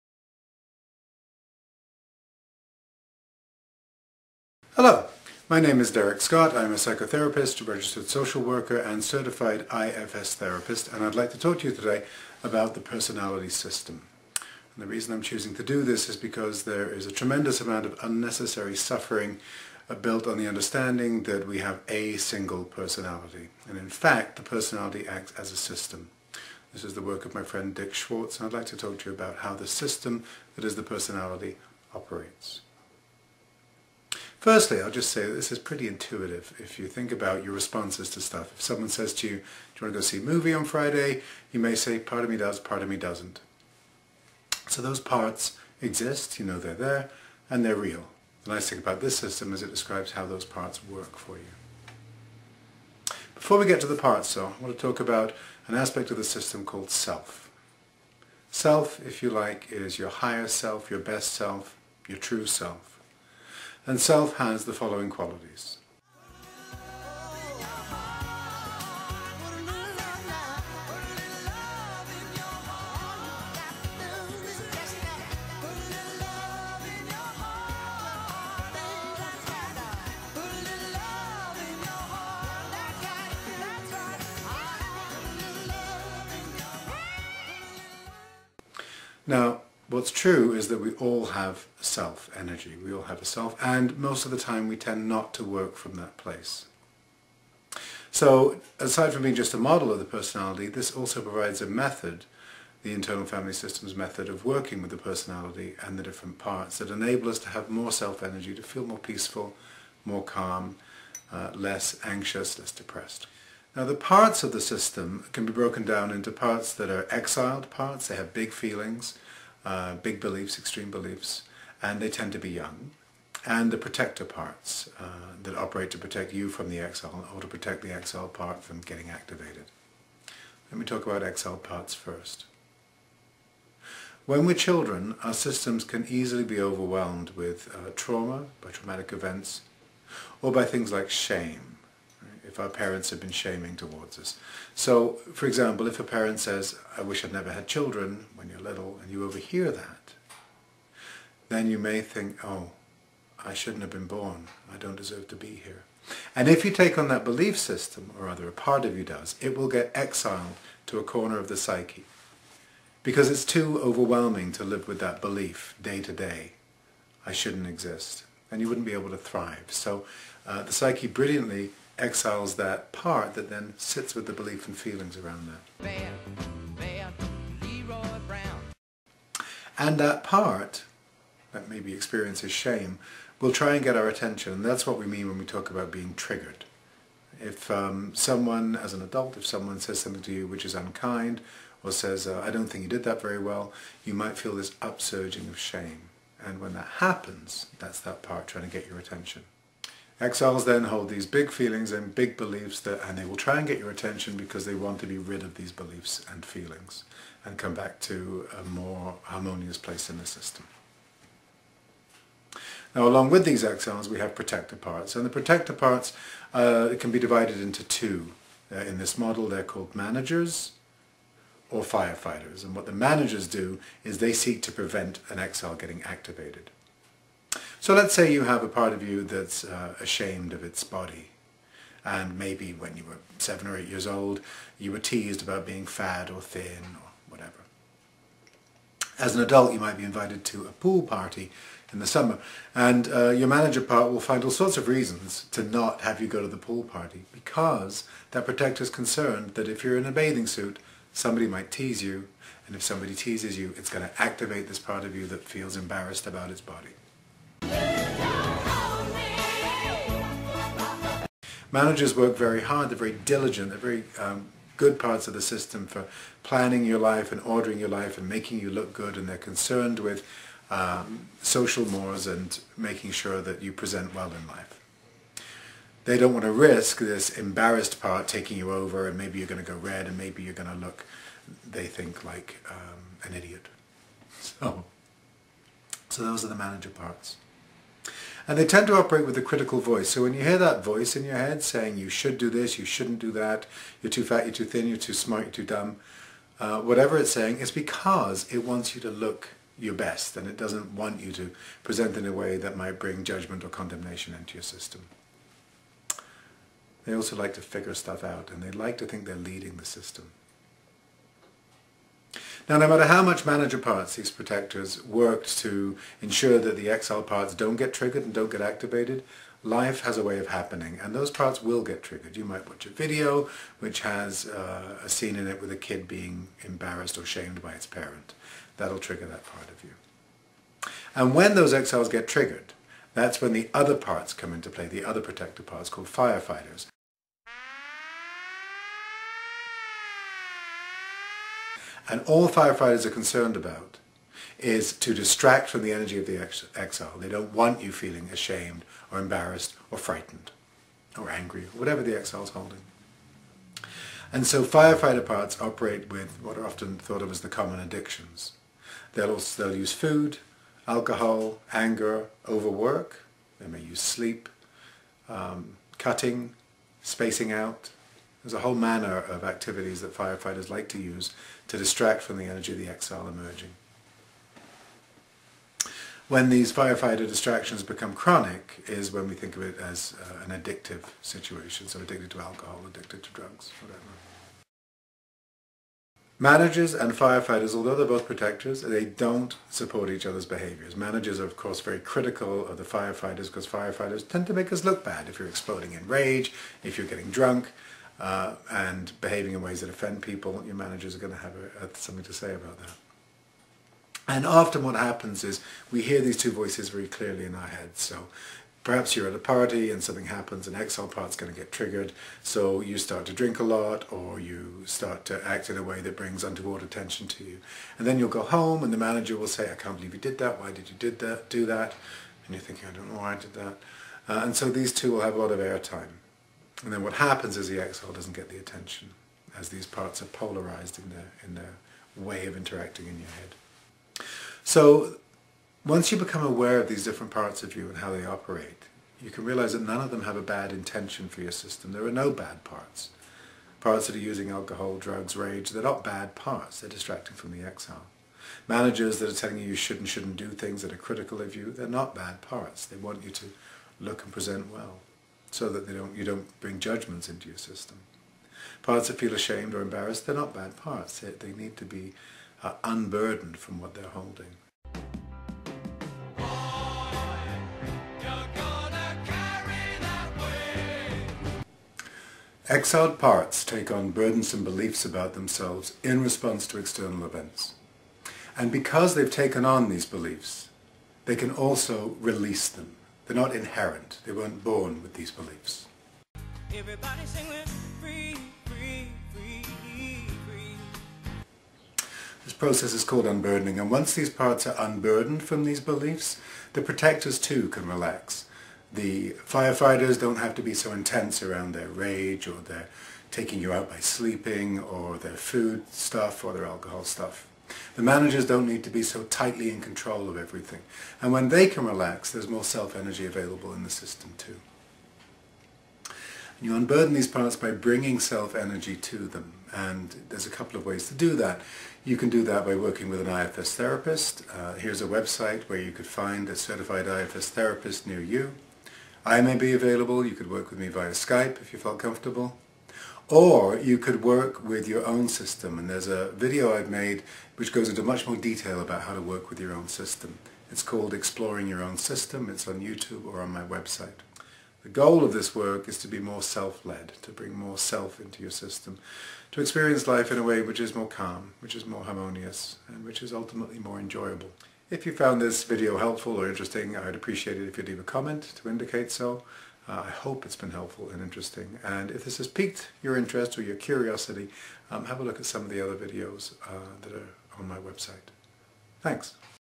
Understanding The Personality System: Introduction to the Internal Family System
Internal Family systems - IFS - the emerging psychospiritual understanding of the personality.
This video describes the pioneering work of Dr. Richard Schwartz in outlining how the personality system operates and how to heal distressed parts by bringing curiousity and compassion from your own Self to them.